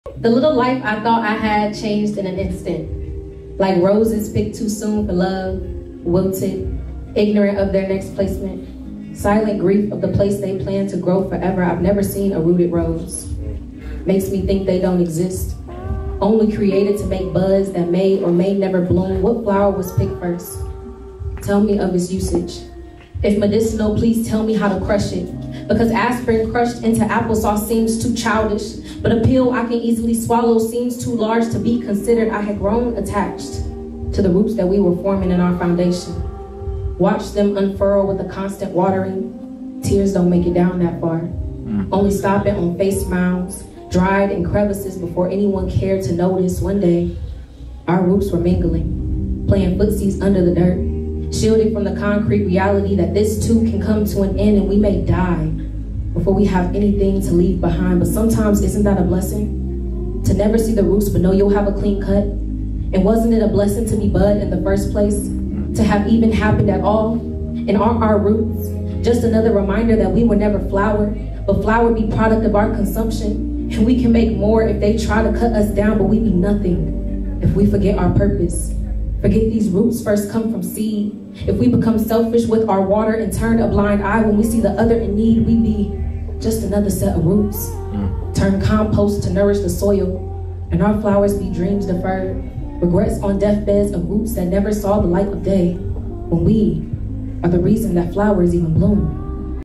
Spoken-Word Poet Alexis Lawson Performs Her Poem "Flowers"
AL.
xo